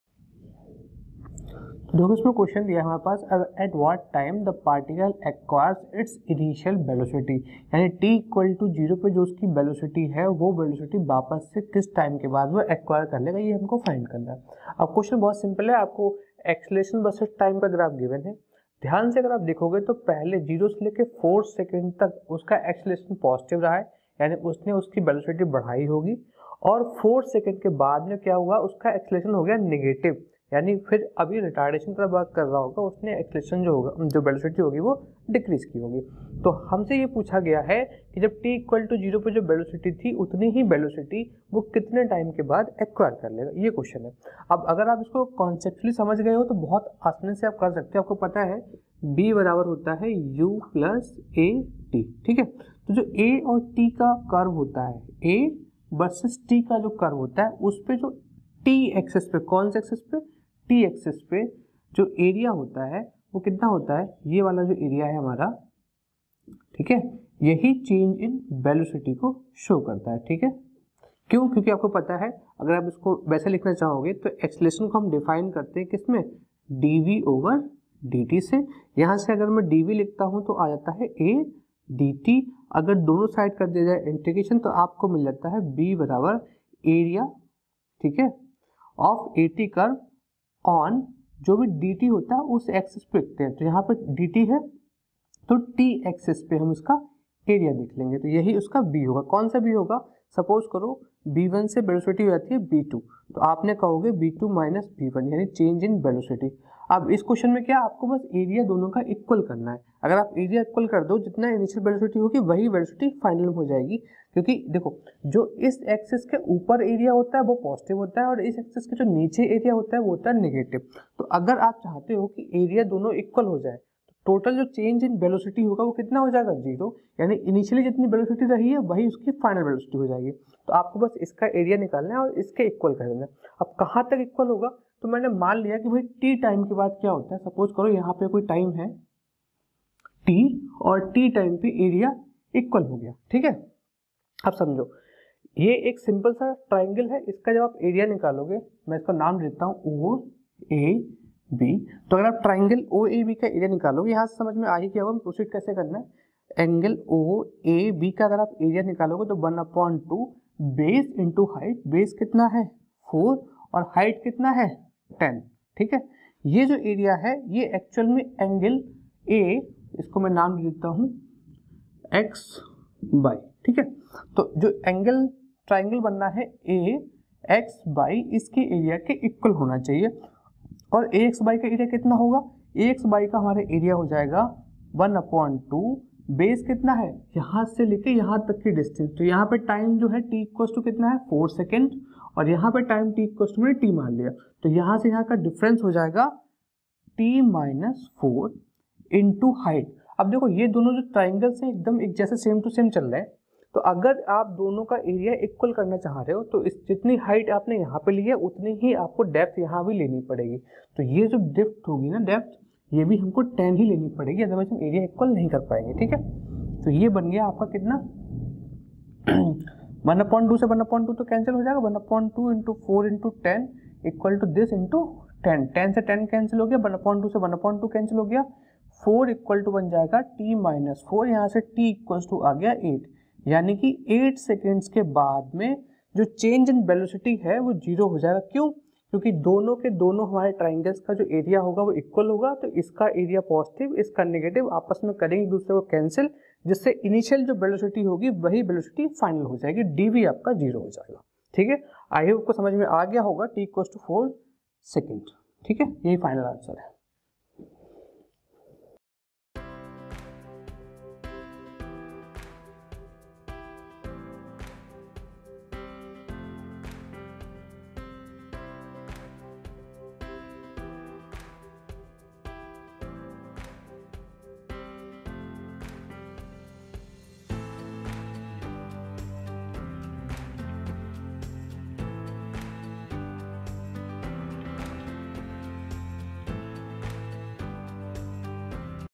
Mechanics Question || a-T graph is given, find initial velocity
Hello, everyone. In this video, I'm going to explain some questions of the EMRS exam and show you why the final answer key provided by EMRS was wrong. The EMRS exam is a national-level entrance test for admission to Eklavya Model Residential Schools, which are residential schools for tribal students in India. The exam was held on 16th December 2023 and the final answer key was released on 9th Jan 2024.

However, many candidates have raised objections to the answer key, claiming that some of the answers were incorrect or ambiguous. I have analyzed the question paper and the answer key and found out that there are at least 7 questions that have wrong or doubtful answers in the answer key. I will go through each of these questions in detail and explain why the answer given by EMRS is wrong and what is the correct answer according to the official syllabus and sources.

You can also find the PDF of this lecture in the Telegram channel